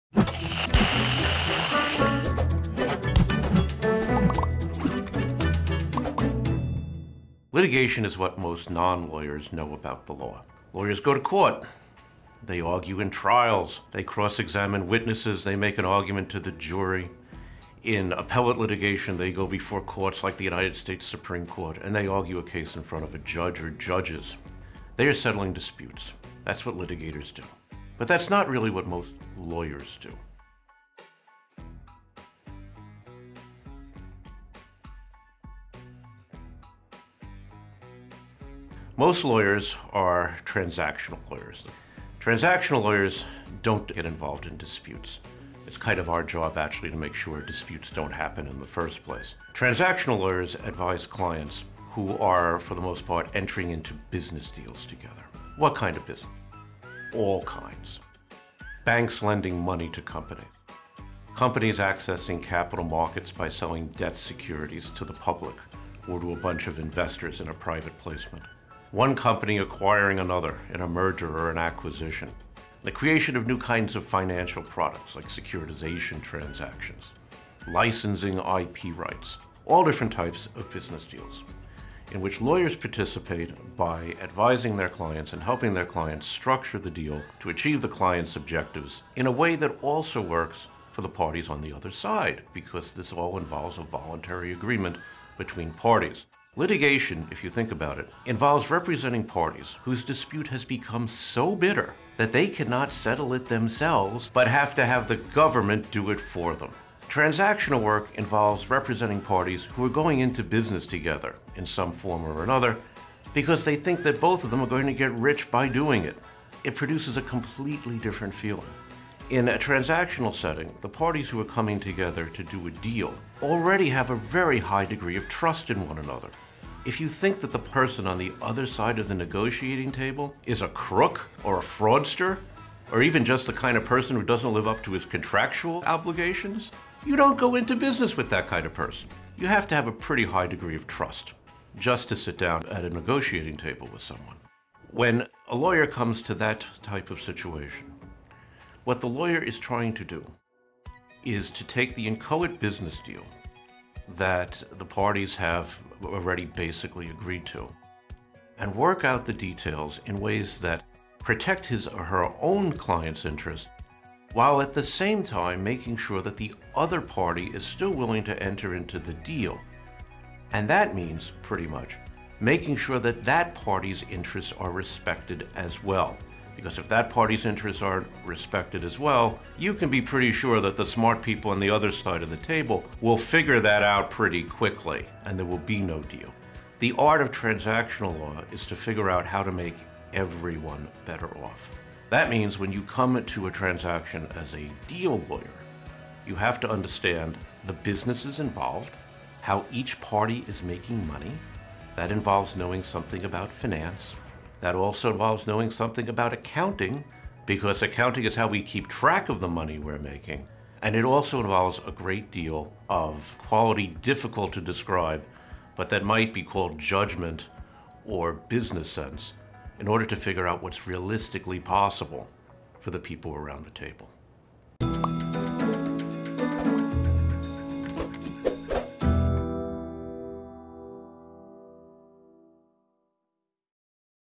What is the Difference between Transactional Law and Litigation? [No. 86]
When the average person thinks about the job of a lawyer, they automatically think about litigation and court cases. Contrary to that impression, the average lawyer is usually involved in legal business transactions. Professor Robert Miller contrasts the two types of legal practice and explains how a lawyer's role and relationship with the client can vary greatly in these different areas.

Robert T. Miller is the F. Arnold Daum Chair in Corporate Finance and Law and the Associate Dean for Faculty Development at the University of Iowa College of Law.